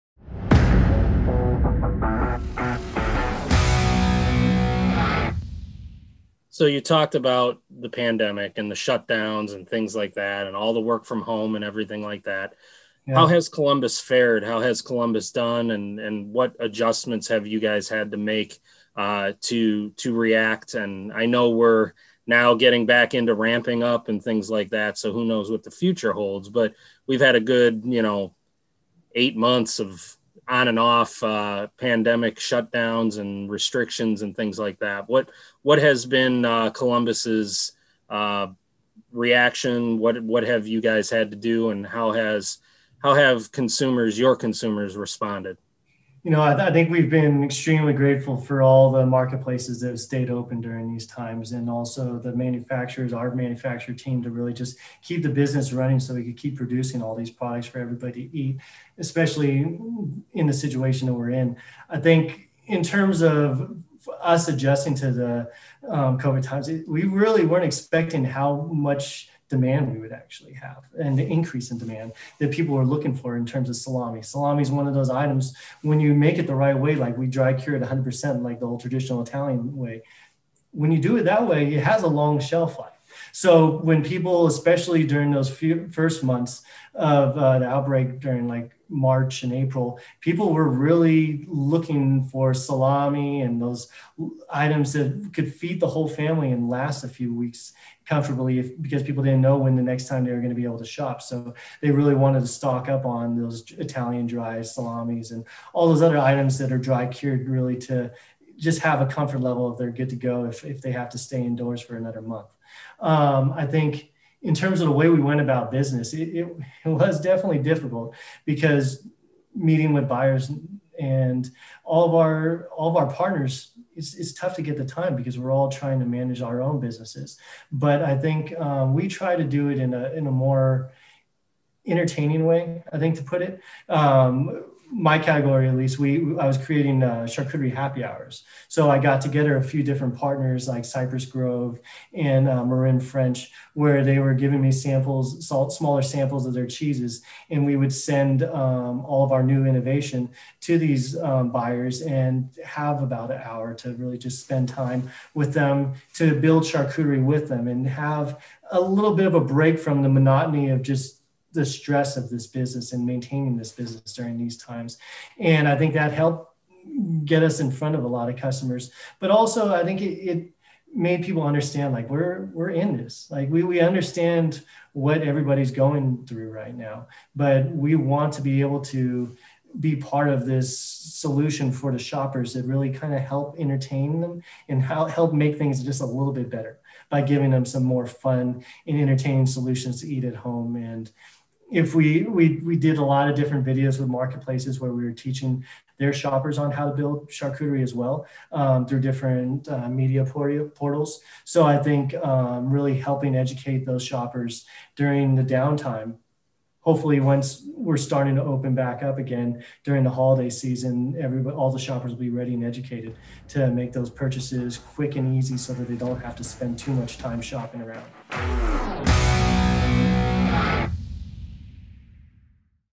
Effect of COVID-19 pandemic on Columbus Craft Meats' business, operations
Andy Hanacek, editor-in-chief, gets an update from Evan Inada, Columbus Craft Meats, on how COVID-19 affected demand for charcuterie and high-end deli proteins.